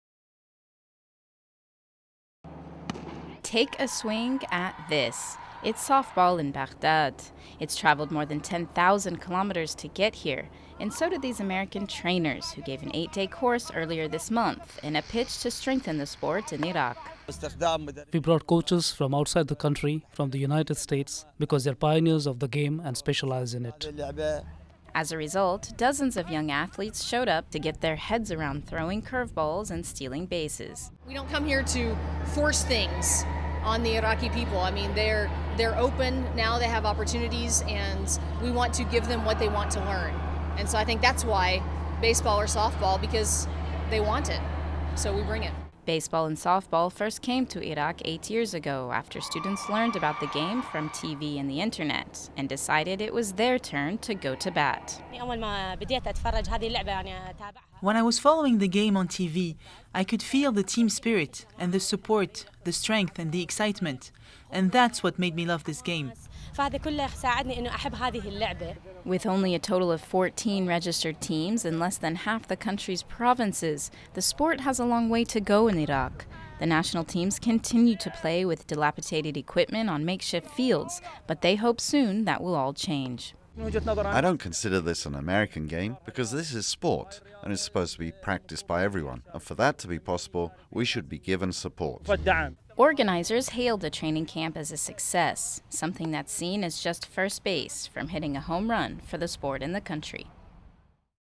Softball in Iraq (2011)
This news segment captures softball that Sport 4 Peace began in Iraq nearly 10 years ago.